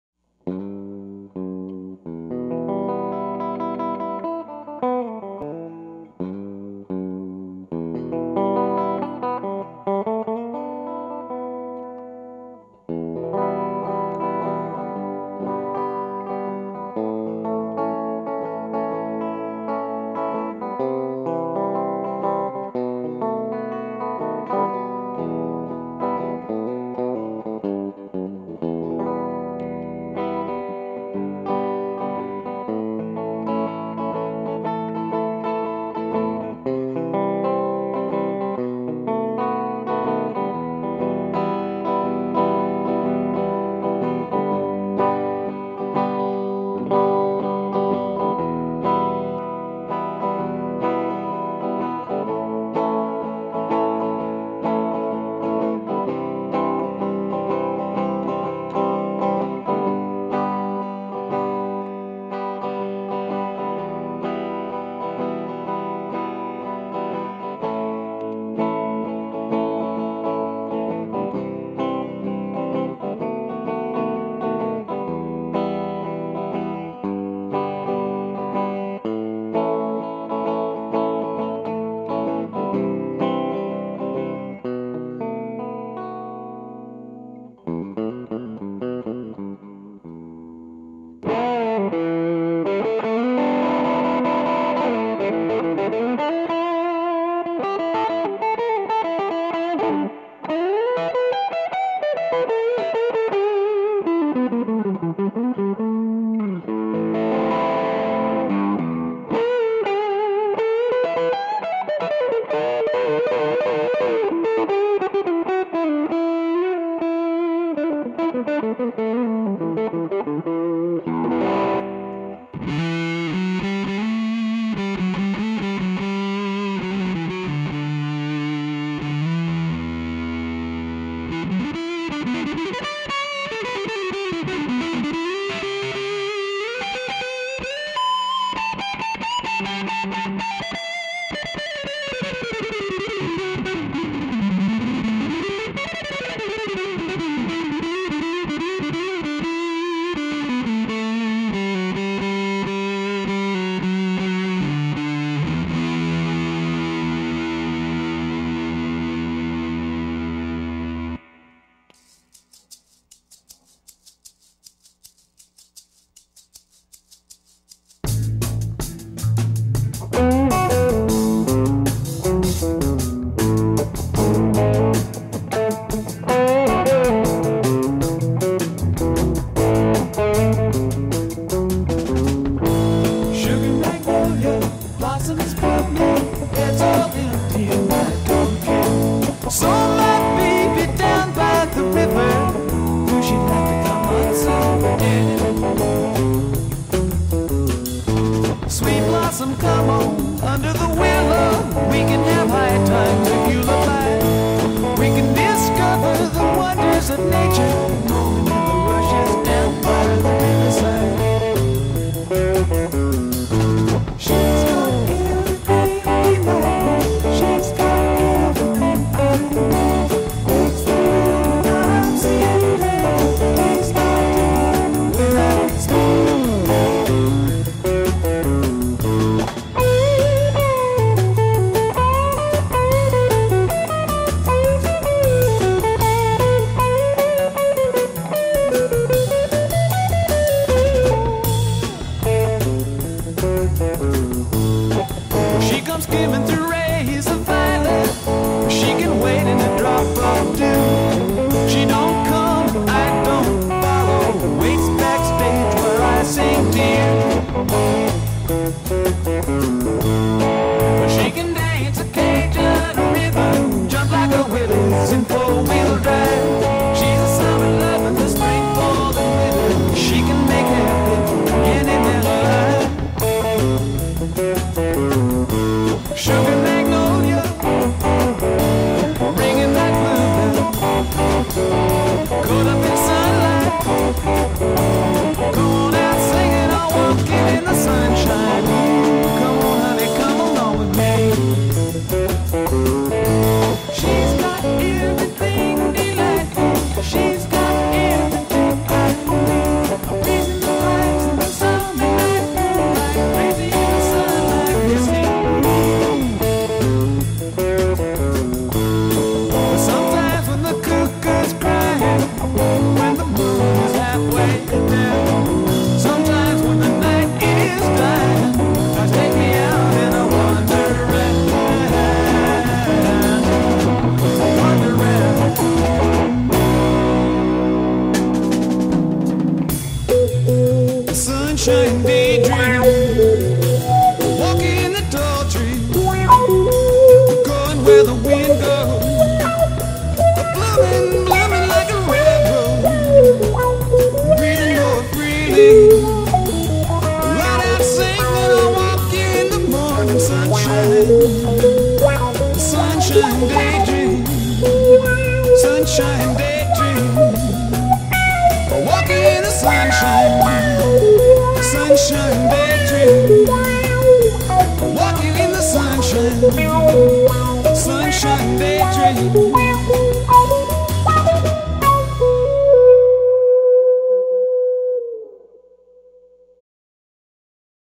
1960s Prestige Japanese Guitar Demo
This model is ultra rare!  I looked around for a long time to find one of these Kawai-made beasts!  Check out that cool body carving!